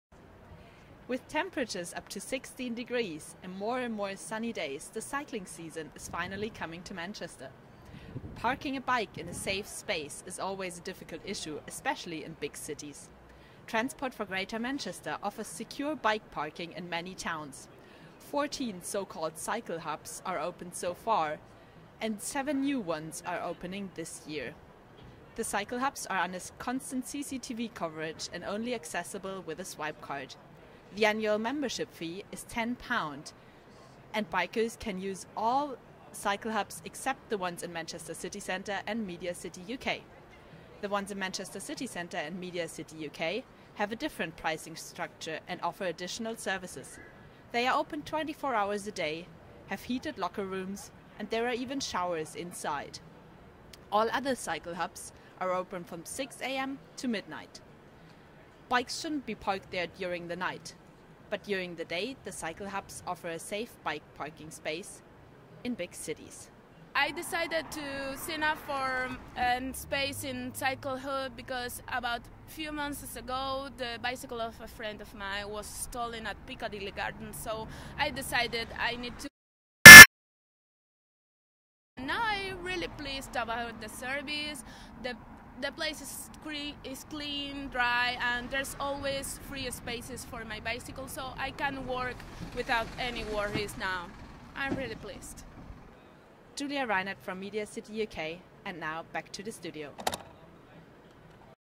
MA International and Online Journalism - TV News Package: Cycle Hubs in the UK
A TV news package about the Cycle Hubs in the UK produced by a MA International and Online Journalism Student in Manchester.